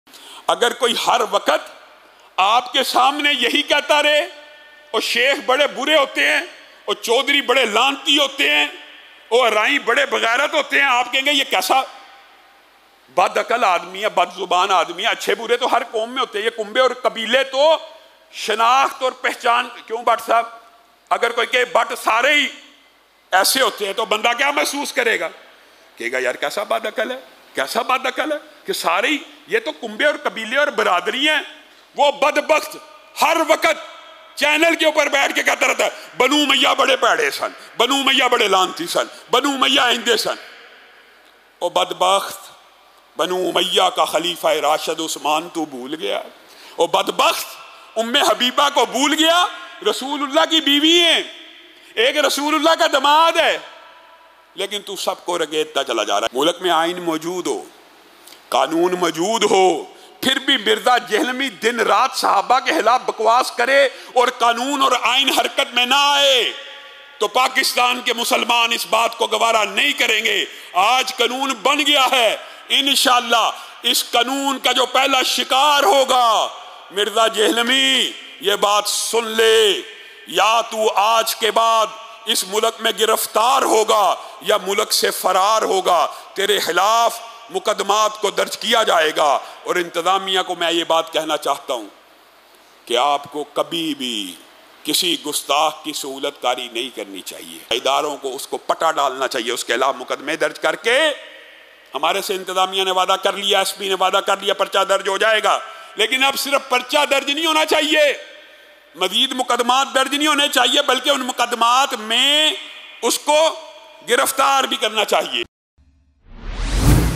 Who was Bannu Umayyah ? Engineer Muhammad Ali Mirza about Bani Umayyah ! Allama Hisham Elahi Zaheer
انجینئر محمد علی مرزا آئے روز بنوامیہ کے خلاف ہرزہ سرائی کرتے ہیں
کیا آپ جانتے ہیں کہ کون کونسی مقدس شخصیات کا تعلق بنو امیہ سے ہے ؟
علامہ ہشام الہیٰ ظہیر کی گفتگو